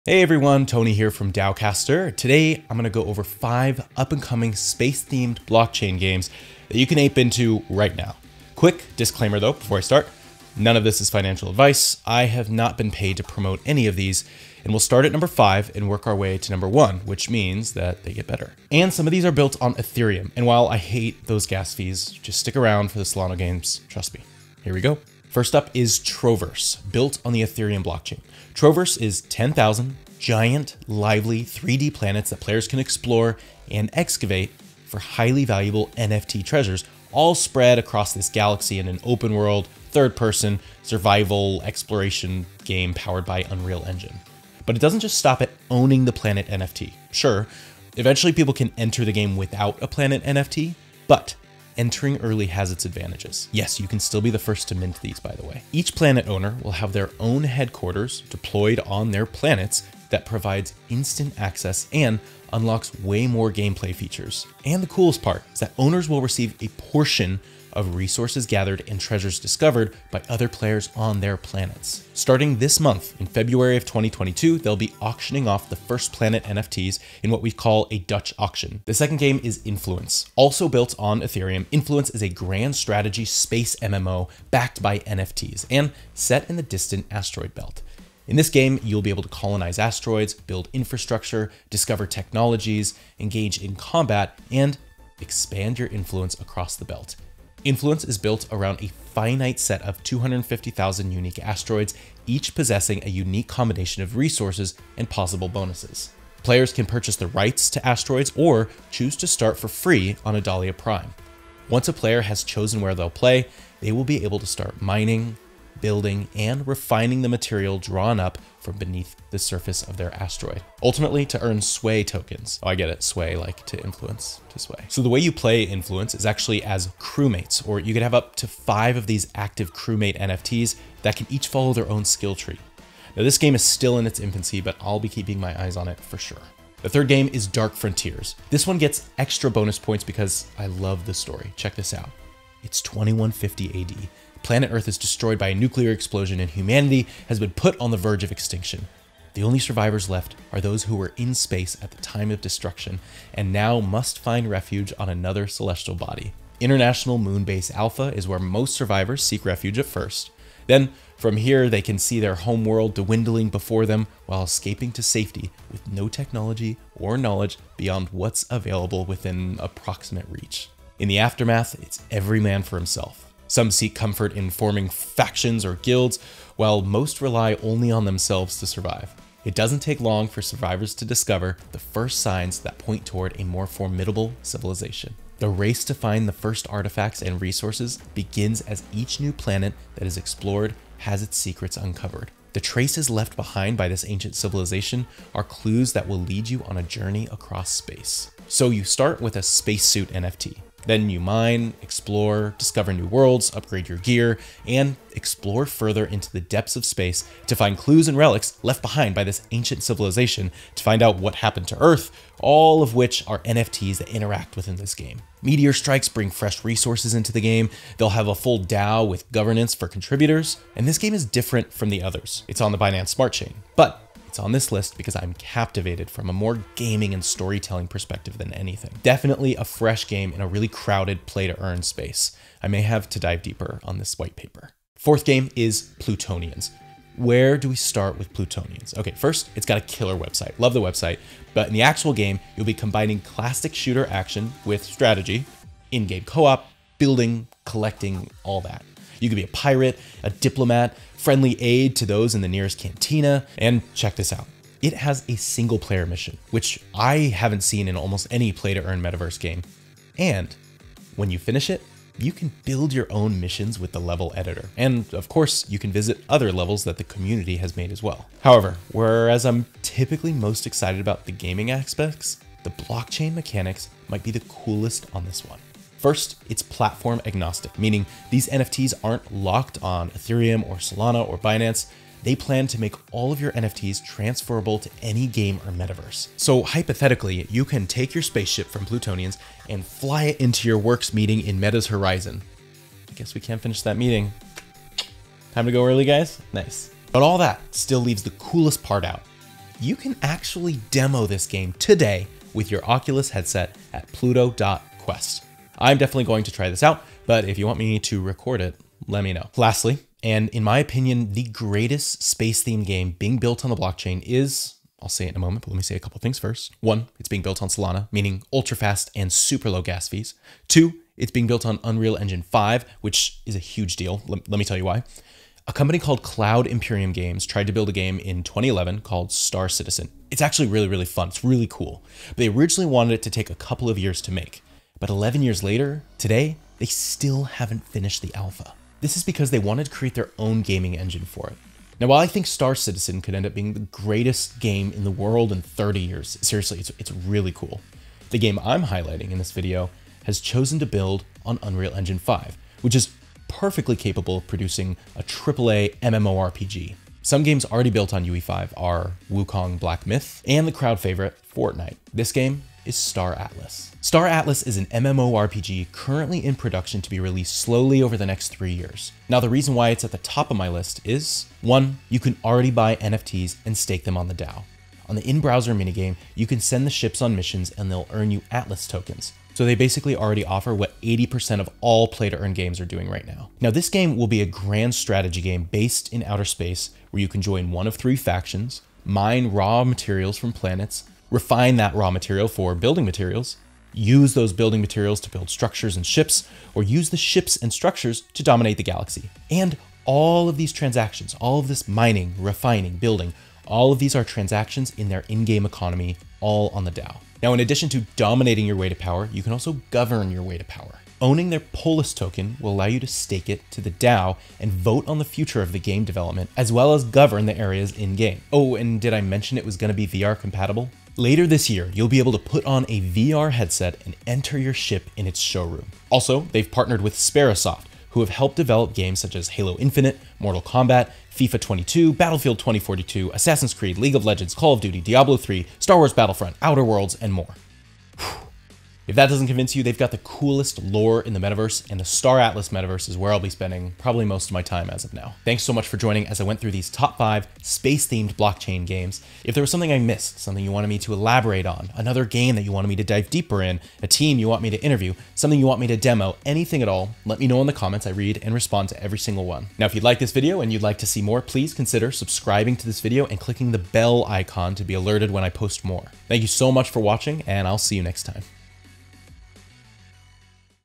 The 5 Best Metaverse P2E Games that Could EXPLODE in 2022: Outer Space Edition
First up is Troverse built on the Ethereum blockchain. Traverse is 10,000 giant, lively, 3D planets that players can explore and excavate for highly valuable NFT treasures all spread all across the Galaxy in the open-world, third-person, space exploration-survival game powered by Unreal Engine. But it doesn’t stop at just owning the planet NFTs, sure, eventually people can enter the game without a planet NFT, BUT, entering early has its advantages. (Yes you can still be first to mint these) Each planet owner will have their own headquarters deployed on their planets that provides instant access and unlocks way more gameplay features AND Owners will receive a portion of resources gathered and treasures discovered by other players on their planets.

Second is Influence. Also built on Ethereum. Influence is a grand strategy space MMO, backed by NFTs, and set in a distant asteroid belt. In this game you’ll be able to Colonize asteroids, build infrastructure, discover technologies, engage in combat and Expand your influence across the belt. Influence is built around a finite set of 250,000 unique asteroids, each possessing a unique combination of resources and possible bonuses. Players can purchase the rights to asteroids or choose to start for free on Adalia Prime.

Third is Dark Frontiers. This one gets extra points because I love the story, check this out. It’s 2150 A.D., planet earth is destroyed by a nuclear explosion and humanity has been put on the verge of extinction. The only survivors left are those who were in space at the time of destruction and now must find refuge on another celestial body.

Fourth is Plutonians. Where do we start with Plutonians. First, it’s got a killer website. But In the actual game, you’ll be combining classic shooter action with strategy, in-game co-op, building, and collecting. You can be a pirate, diplomat, friendly aid to others in the nearest cantina. And check this out, it has a single player mission, which I haven’t seen in almost any P2E metaverse game, AND, when you finish it, you can build your own mission with the level editor and of course visit other levels that the community has made.

Lastly, in my opinion, the greatest space-themed game being built on the blockchain is…
Being built on Solana, meaning ultra fast and super low gas fees, it’s being built in Unreal Engine 5, which is a HUGE deal. I’ll tell you why. A company called Cloud Imperium Games tried to build a game in 2011 called Star Citizen. They originally wanted it to take a couple of years to make, but 11 years later, they still haven’t finished the alpha. This is because they wanted to create their own gaming engine for it. While I think Star Citizen could end up being the greatest game in the world in 30 years, the game I’m highlighting in this video has chosen to build on Unreal Engine5, which is PERFECTLY capable of producing a triple-A MMORPG. Some game already built on UE5 are Black Myth: Wukong and the crowd favorite, fortnite. This game is Star Atlas.  Star Atlas is an MMORPG in currently in production to be released slowly over the next 3 years. The reason it is on the top of this list, is…. ONE,  you can already buy NFTs and stake them on the DAO. On the in-browser mini-game, you can send the ships on missions and they’ll earn you $ATLAS tokens. So, basically 80% of all of the other fully developed Play2Earn games are doing. This games will be a grand strategy game based in outer space, where you can join 1 of three factions, mine raw materials from planets, refine that material for building materials, use those building materials to build structures and ships, or use ships and structures to dominate the galaxy. Additionally to battling your way to power, you can also govern your way to power. Owning their $POLIS token will allow you to stake it to the DAO and vote on the future of the game development as well as govern the areas in-game. Oh, and did I mention it will be VR compatible? Later this year, you’ll be able to put on a VR headset and enter your ship in it’s showroom.  Also, they’ve partnered with Sperasoft, who have helped develop games such as Halo Infinite, Mortal Combat, FIFA22, Battlefield 2042, Assassin’s Creed, League of Legends, Call of Duty, Diablo 3, Star Wars Battlefront, Outer worlds and more.